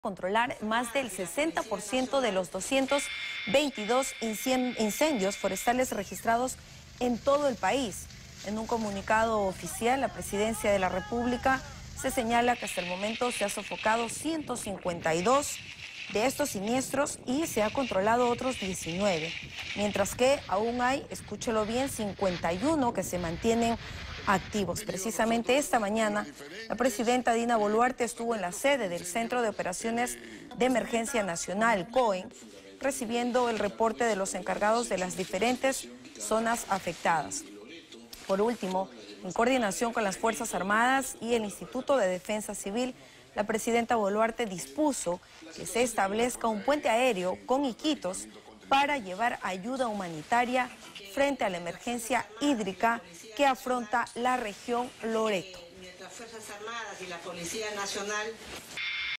Establecen puente aéreo para llevar ayuda humanitaria a Loreto
A través de un comunicado, el Gobierno de la presidenta Dina Boluarte informó que hasta la fecha ha logrado controlar más del 60 por ciento de los 222 incendios forestales registrados en todo el país.

A través de redes sociales, Presidencia de la República señaló que hasta el momento se ha sofocado 152 incendios y se ha controlado otros 19, mientras que aún hay 51 que se mantienen activos.

Precisamente, este sábado la presidenta Dina Boluarte estuvo en la sede del Centro de Operaciones de Emergencia Nacional (COEN) recibiendo el reporte de los encargados de las diferentes zonas afectadas.

AYUDA PARA LORETO

En coordinación con las Fuerzas Armadas y el Instituto de Defensa Civil, se dispuso que se establezca un puente aéreo con Iquitos para llevar ayuda humanitaria frente a la emergencia hídrica que afronta la región Loreto.